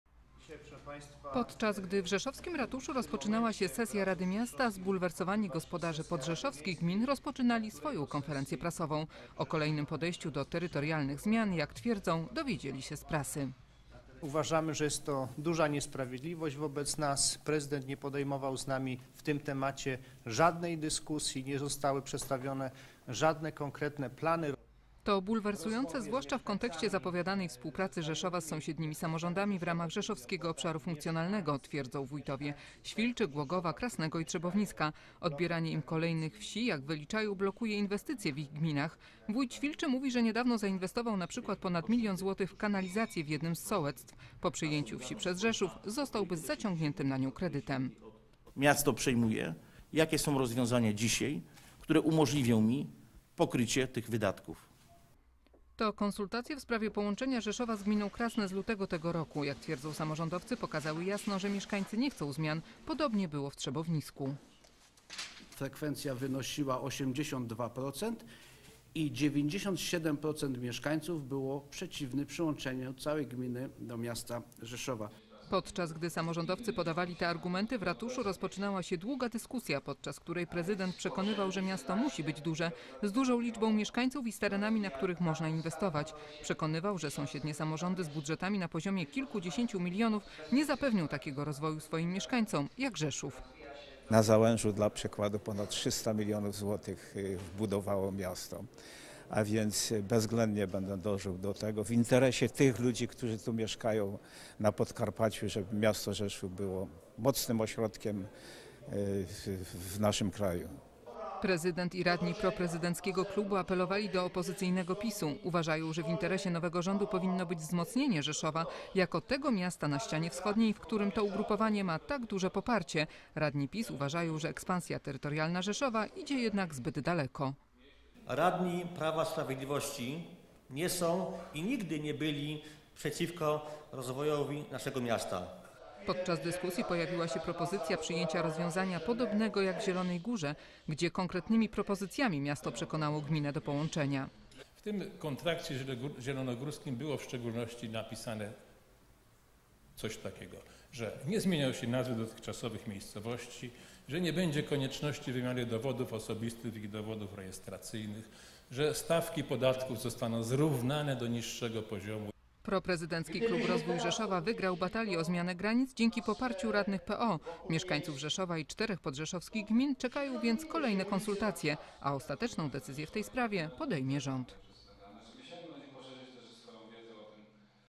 Rzeszów kontra okoliczne gminy - nowa wojna o granice
Jeszcze nie wybrzmiały echa poprzednich, a już szykują się kolejne konsultacje społeczne w sprawie granic Rzeszowa. Odbędą się w stolicy Podkarpacia i ościennych gminach: Świlcza, Trzebownisko, Głogów Małopolski i Krasne. To skutek dzisiejszych uchwał Rady Miasta Rzeszowa. Jedna to propozycja połączenia miasta z gminą Krasne. Druga - to włączenie do miasta 12 sołectw albo ich części. Samorządowcy z podrzeszowskich gmin nazwali to arogancją władz Rzeszowa. Prezydent miasta mówi o budowaniu silnej stolicy Podkarpacia.